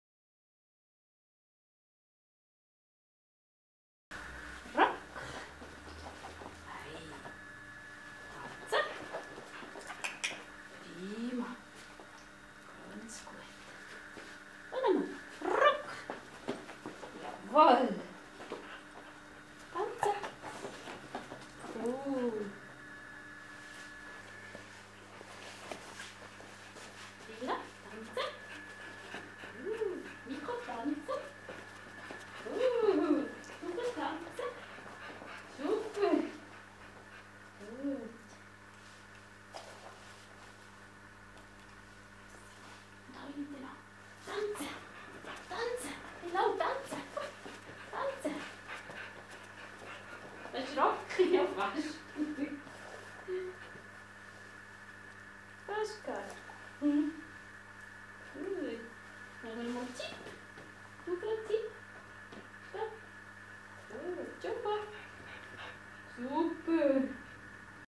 Lappis üben das Drehen auf dem Hocker - zu zweit und zu dritt
Gar nicht so einfach, dass alle miteinander drehen. Übung macht den Meister!!